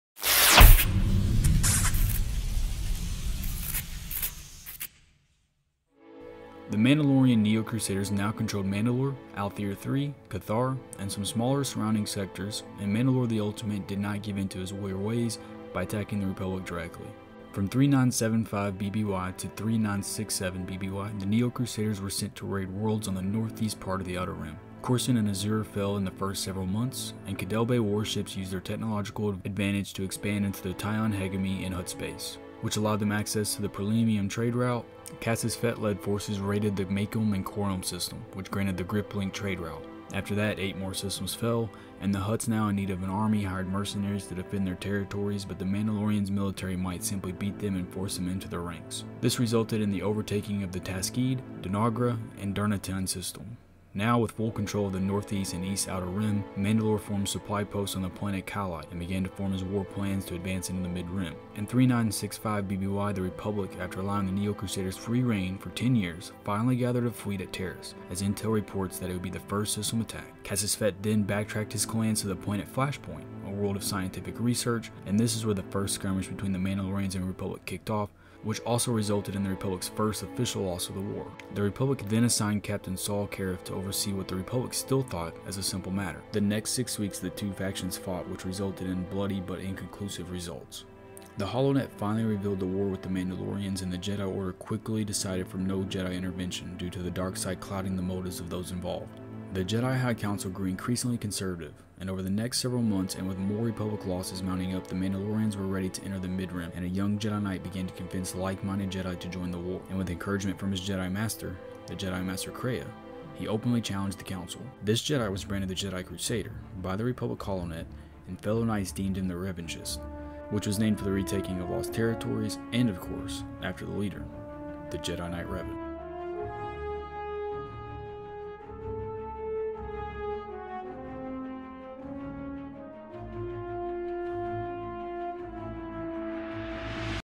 Star Wars Chronology. The Mandalorian War. Part 2. The Outer Rim Sieges
Expanded/Legends. In this episode of Star Wars Chronology we cover Mandalore The Ultimates war plan on the outer rim and how does the republic react to this? The Jedi finally catch wind of the Neo Crusaders but to much dismay elect not to intervene. However a young Jedi rises up.

bensounds. com for the music!